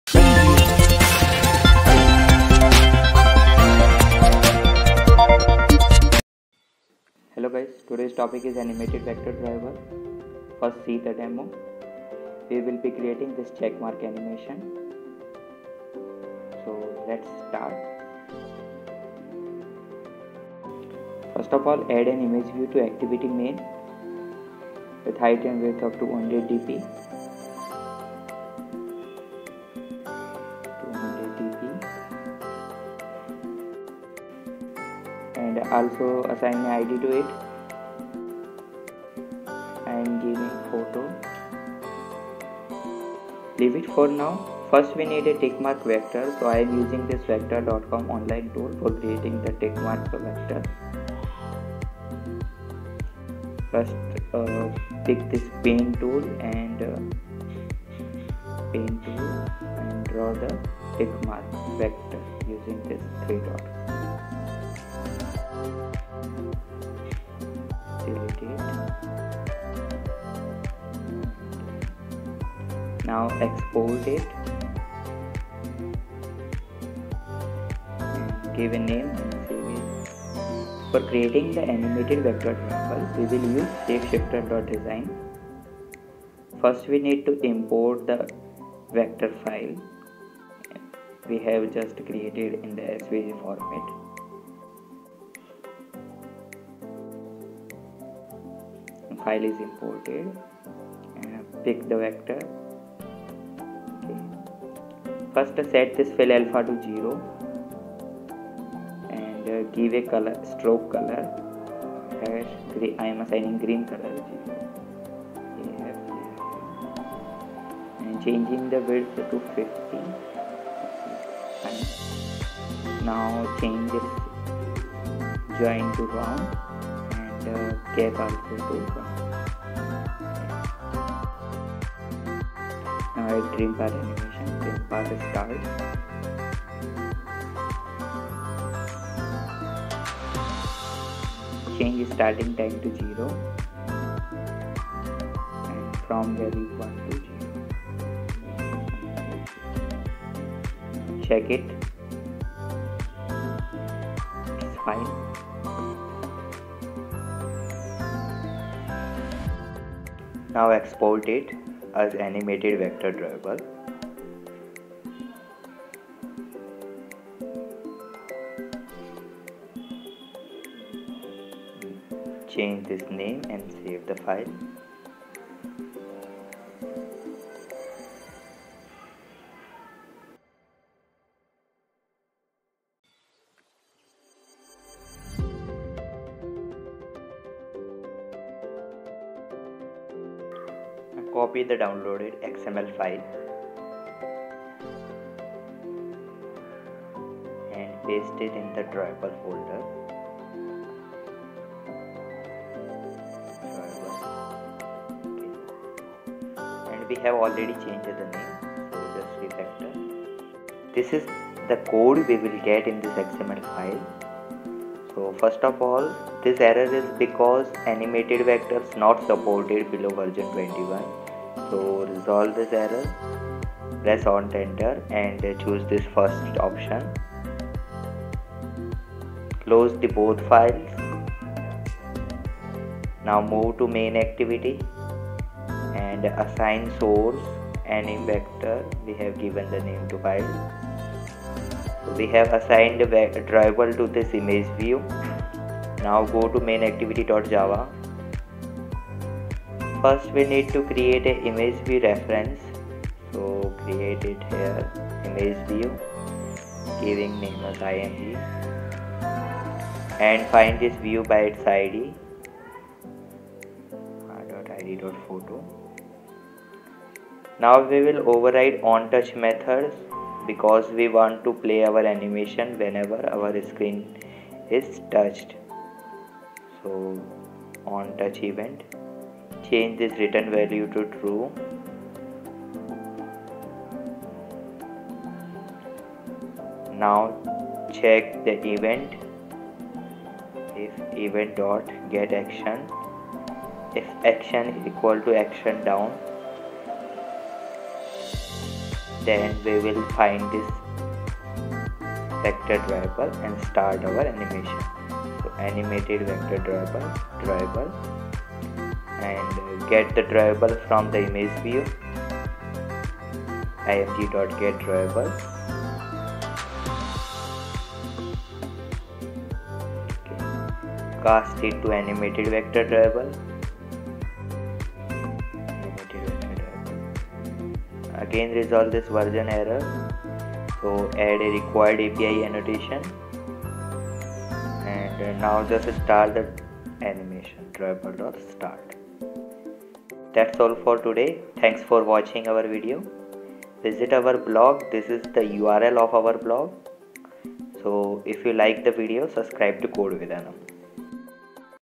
Animate Vector Drawable in Android |Tick Mark Animation
In this tutorial we will see how to create a animated vector drawable in android studio. We will create check mark animation (tick mark animation).

Musician: @iksonmusic